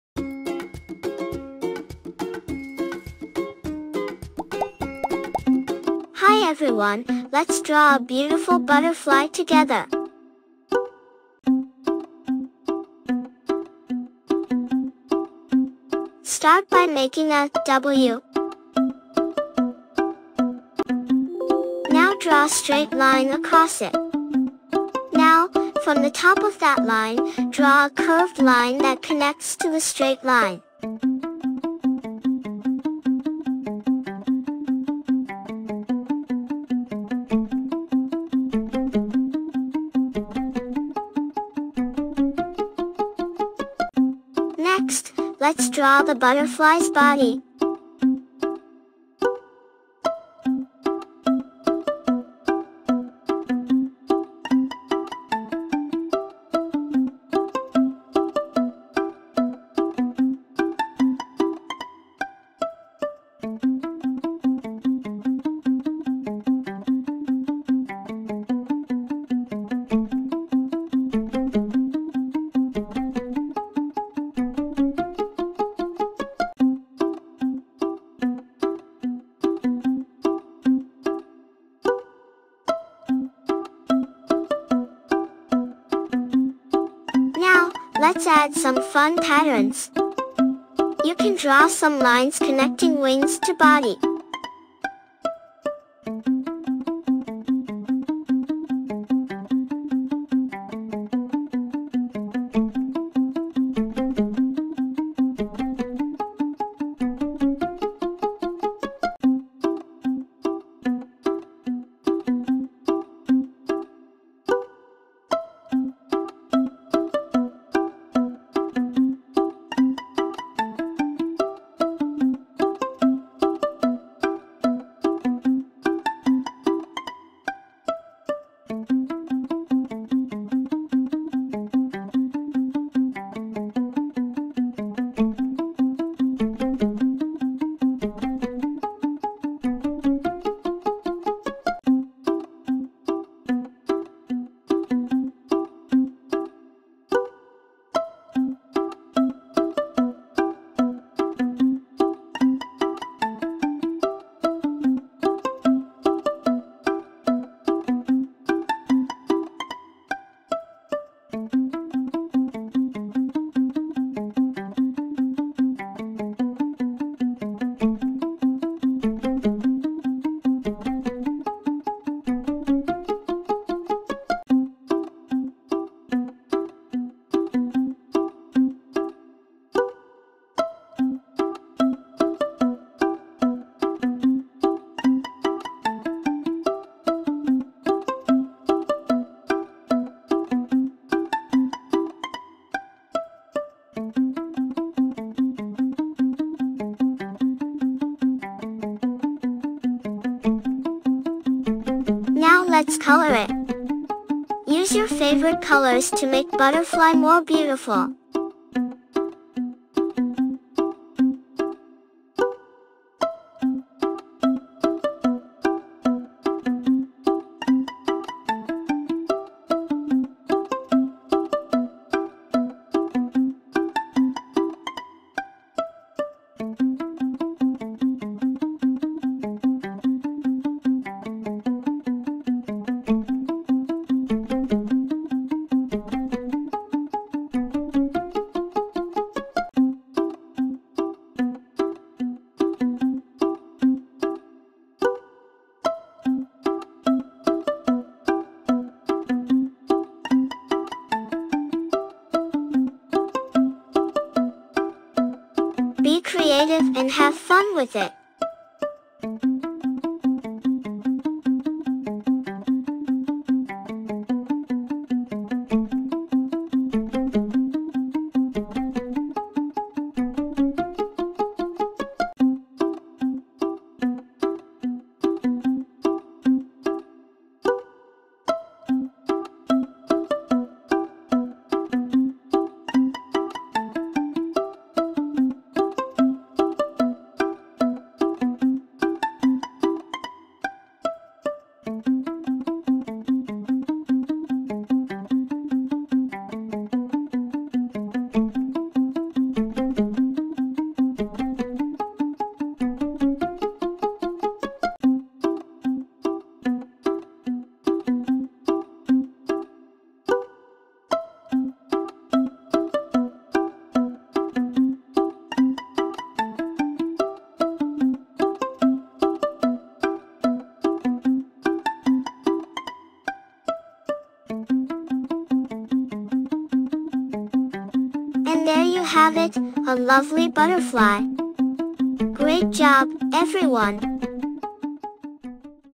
🦋 Let's Draw a Butterfly! 🎨 Fun and Easy Kids' Art Tutorial
Welcome to our colorful world of art! In this video, we'll learn how to draw a beautiful butterfly step by step. It's super easy and perfect for kids of all ages!

First, we'll start with a horizontal line as the butterfly's body. Then, we'll add wings by drawing curved lines that connect to the body. After that, we'll create the butterfly's cute little body right in the middle.

But wait, the fun doesn't stop there! We'll also add our own unique patterns to the butterfly's wings using our favorite colors. Whether you want to make dots, stripes, or hearts, it's all up to your imagination!

So grab your paper, pencils, and crayons, and let's dive into this fun art adventure together. 🌈✨